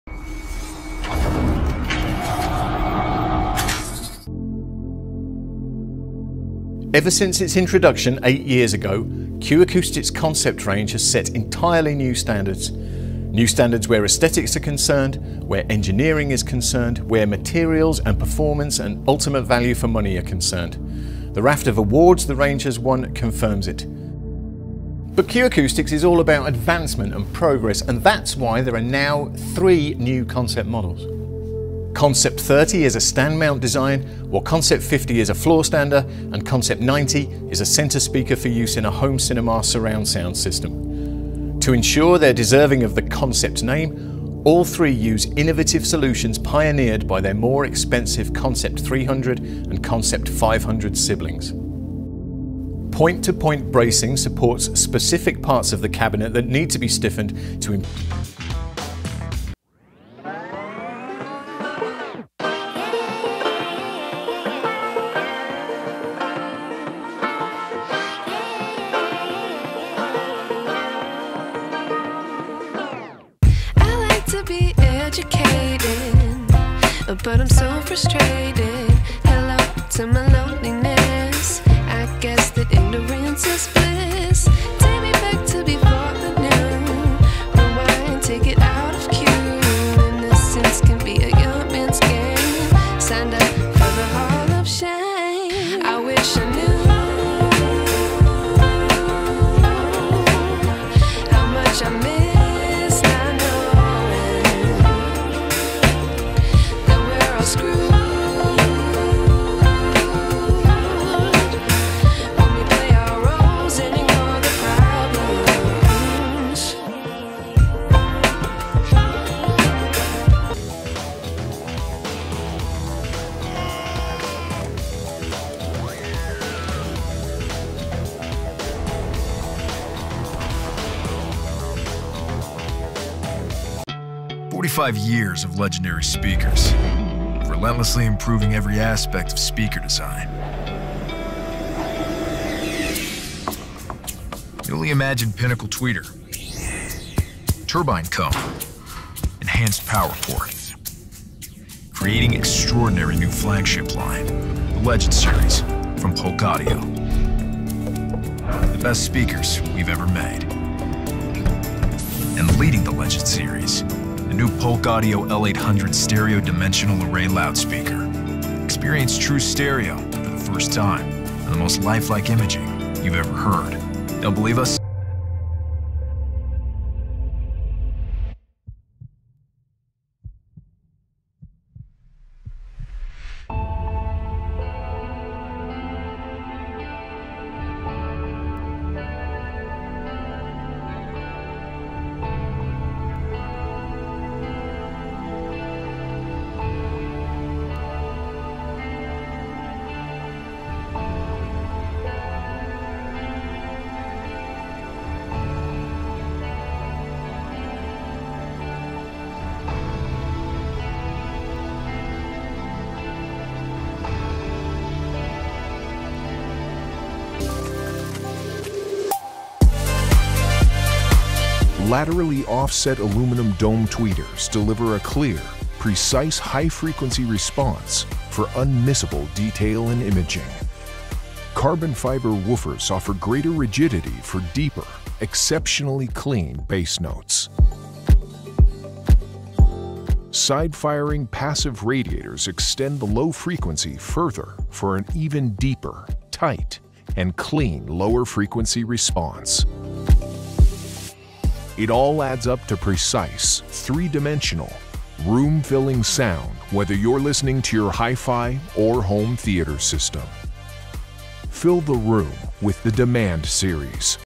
5 BEST FLOOR STANDING SPEAKERS 2023
5Q Acoustics New Concept 30 & 50 & 90
►Amazon US

4 Klipsch Reference 2022
►Amazon US

3️Polk Legend SDA Stereo Mix
►Amazon US

2️Q Acoustics Concept 500 Floorstanding Speakers
►Amazon US

1️Definitive Technology D17 Demand Series Modern High-Performance 3-Way Tower Speaker
►Amazon US